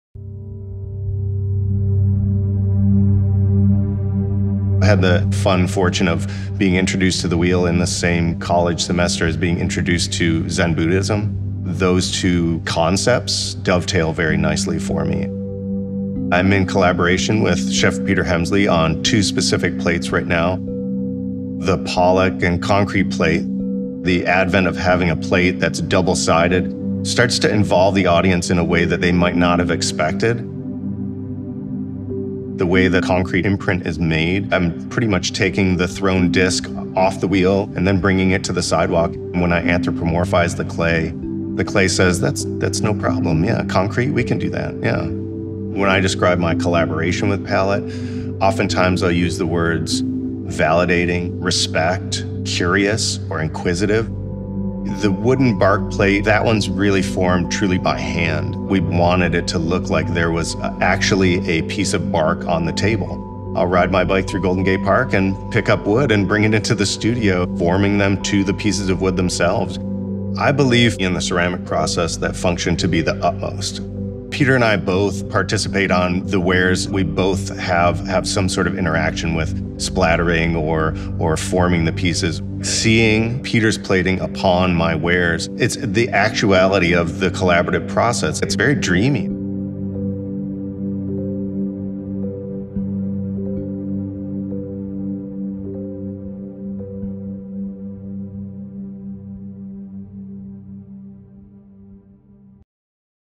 Palette Artisan: Ceramist Andrew Kontrabecki
Andrew Kontrabecki believes good functionality reaches beyond the function of wares, often changing the environment the ceramics are used in. His wares are imbued with a reductive style that isolates the purpose of the object while eschewing additional flourish and ornament, depicting the beauty of an object as it is and finding style in how it might interact with its surroundings.

As house ceramist, Andrew leads the ceramics program at Palette, creating wares for the restaurant and Shop. Chef Peter and Andrew work together seamlessly—as Peter envisions dishes for the menu, ceramic wares are designed simultaneously.